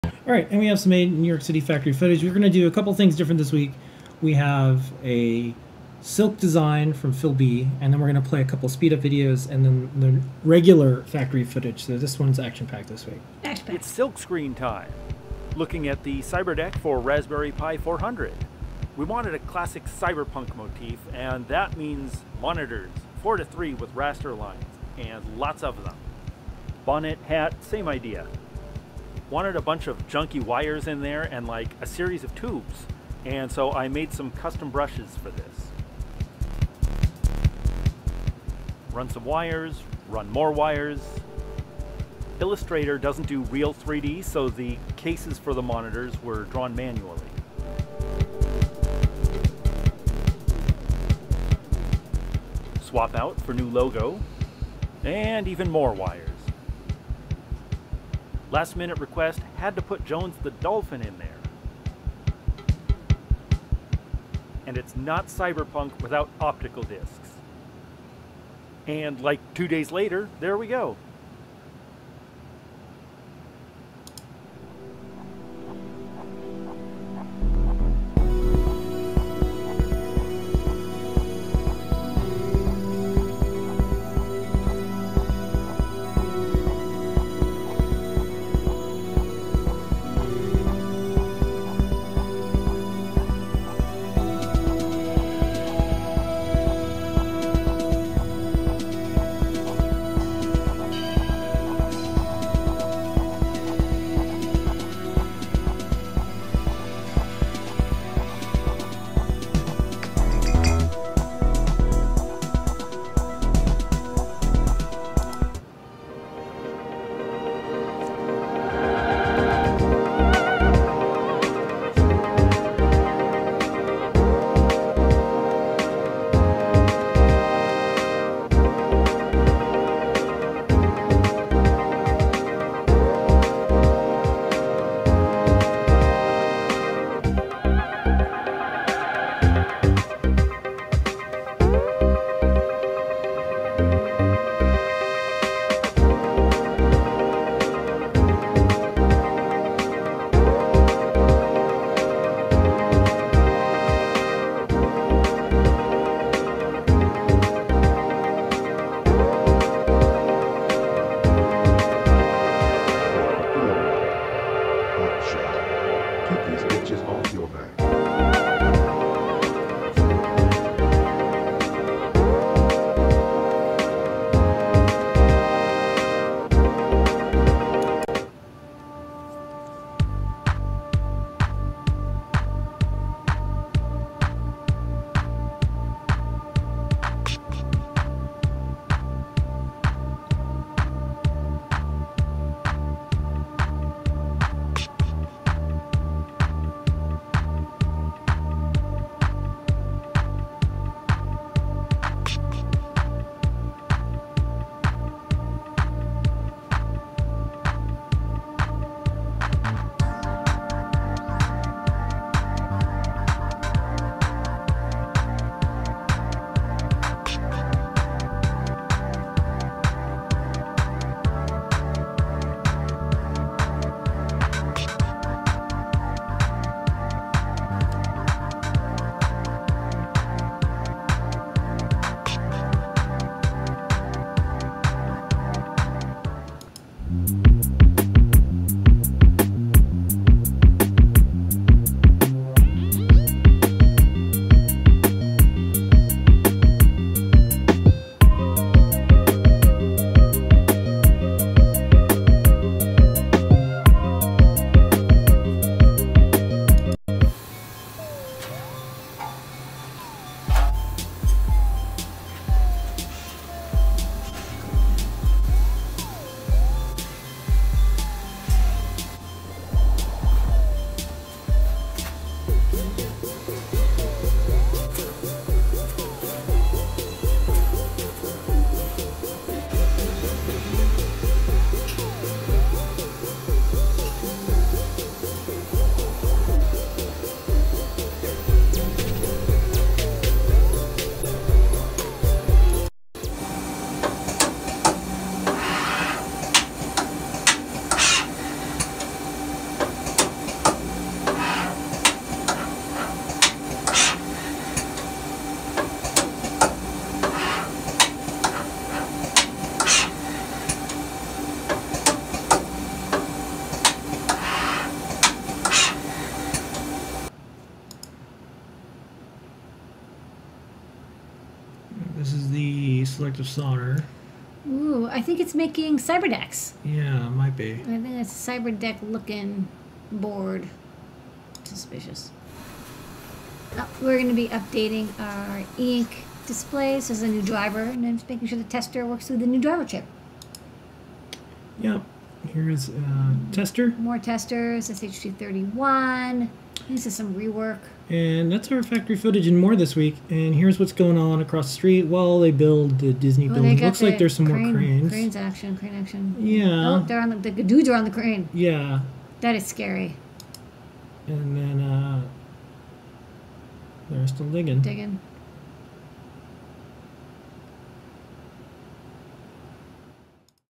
Made in NYC 3/31/2021 Featuring CYBERDECK Silkscreen Design, RP2040 Experiments, PyRulers & More!
Phil B’s CYBERDECK Silkscreen design process (0:14)

DVI Breakout friend for RP2040 video experimentation time lapse (1:14)

TCA4307 Hot Swappable I2C Bus Buffer with Stuck Bus Recovery assembly time lapse (1:56)

Adafruit ItsyBitsy RP2040 prototype assembly time lapse (2:59)

Teensy tester assembly (3:57)

Making PyRulers (4:17)

Stencil creating support (4:53)

Selective solder machine working on USB jacks (5:18)

Testing e-ink display (5:32)

Tester for SHT31-D Temperature & Humidity Sensor (5:40)

Burnt thermocouple amplifier chip (5:45)

Crane being constructed (5:48)

Concrete being poured (6:00)